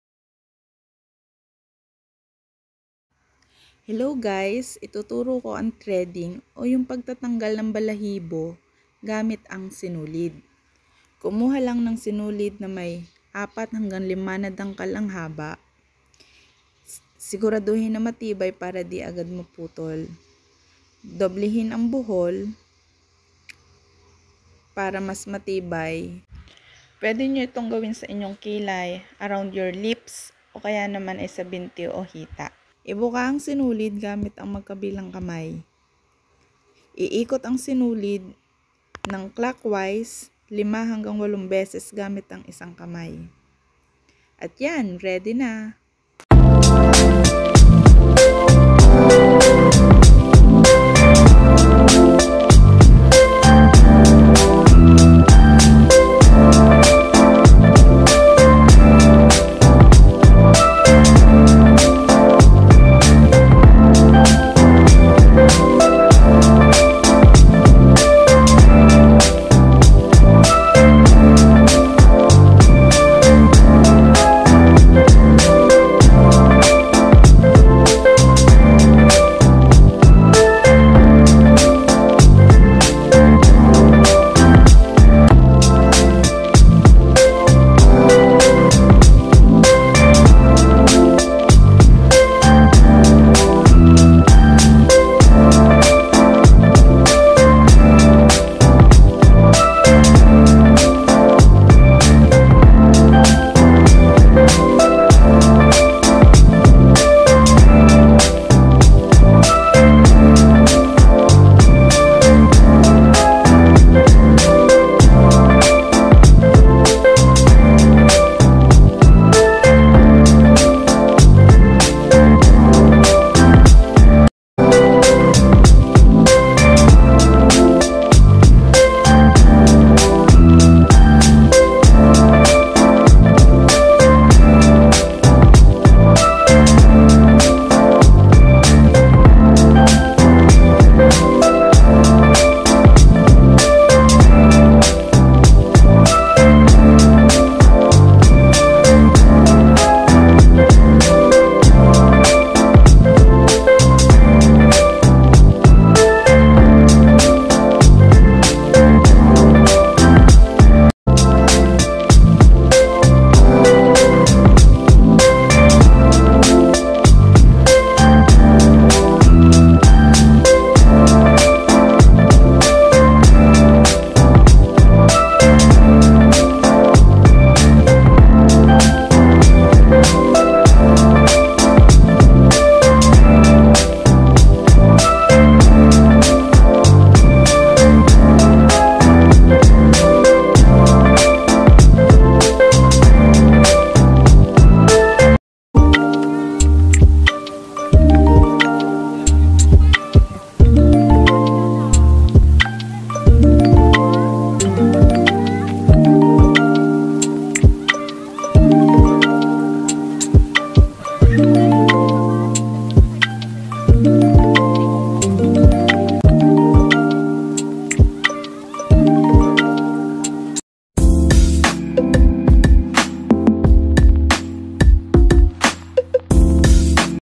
DIY: Threading Tutorial/ Remove Unwanted Hairs/ The cheapest way of hair removal
I remove unwanted hairs  "around my lips" and to my legs only.
This is the cheapest and simplest way that I can recommend to all of my viewers and subscribers. I used THREADING for about 7-8 years. It is important for me to learn how to do it because the hair will grow back again. Base on my experienced, the hair around my lips grow just few days after the threading. Unlike on my legs that it took 2 weeks up to 1 month before the hair removal. To less the pain I put powder or body lotion before threading, somehow it helps for me. But actually it don’t hurt me anymore.